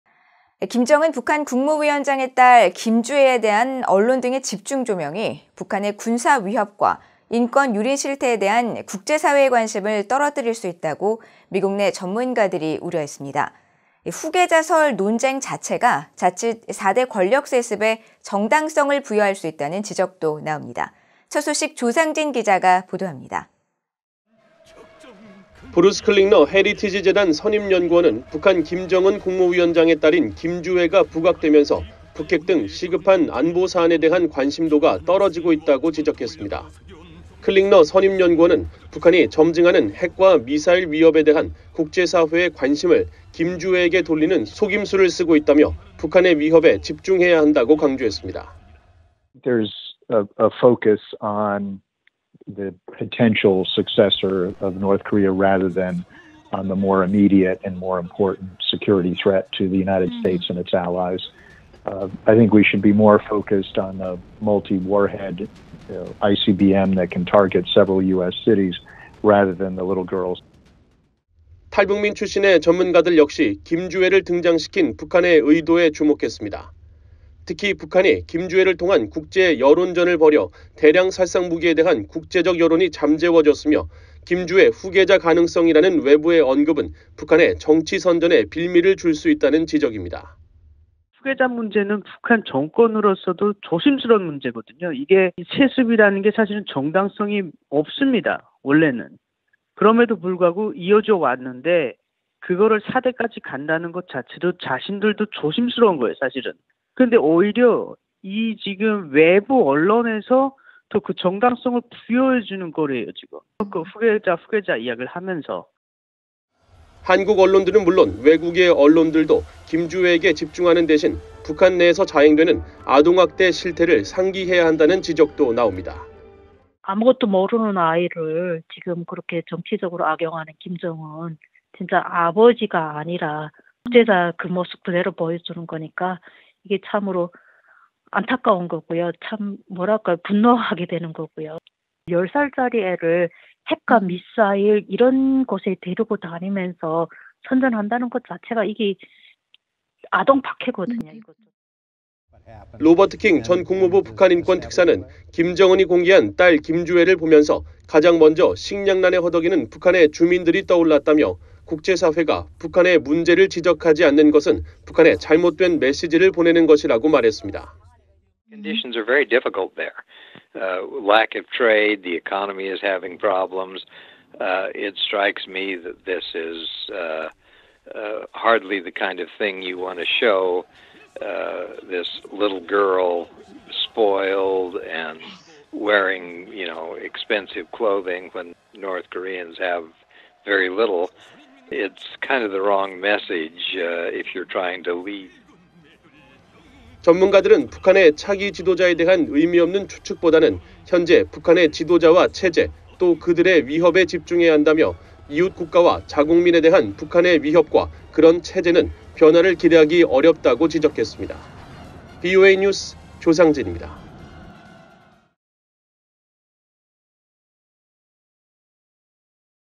[VOA 뉴스] 김주애 ‘과도한 조명’…‘북핵 위협·인권 유린’ 경시
김정은 북한 국무위원장의 딸 김주애에 대한 언론 등의 집중 조명이 북한의 군사 위협과 인권 유린 실태에 대한 국제사회의 관심을 떨어뜨릴 수 있다고 미국 내 전문가들이 우려했습니다. 후계자설 논쟁 자체가 자칫 4대 권력세습에 정당성을 부여할 수 있다는 지적도 나옵니다. 조상진 기자가 보도합니다. (영상편집: 김정호)